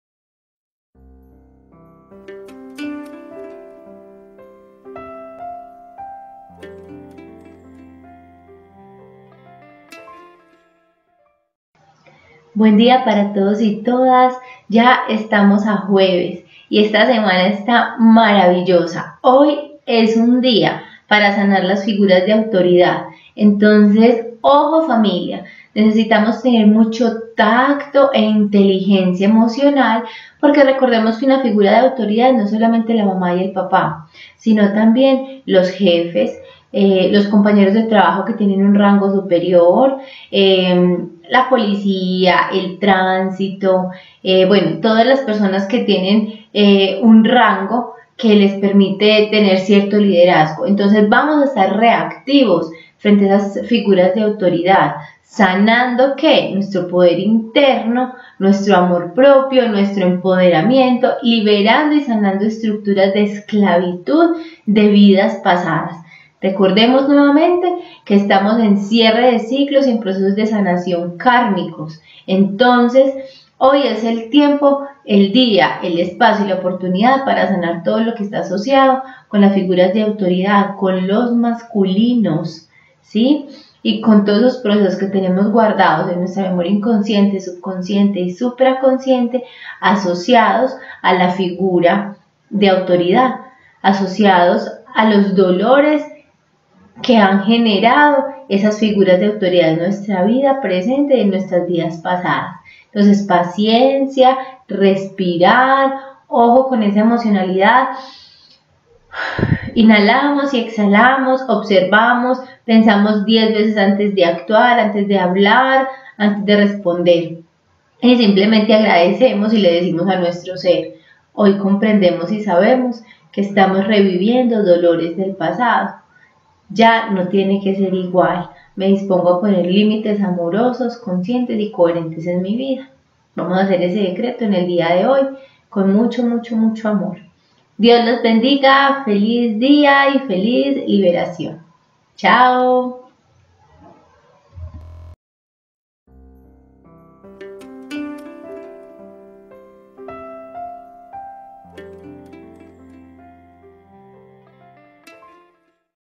Mensaje diario de los ángeles
¡Buen día a tod@s! hoy los ángeles nos invitan a sanar figuras de autoridad, y a poner límites amorosos y conscientes en nuestras vidas.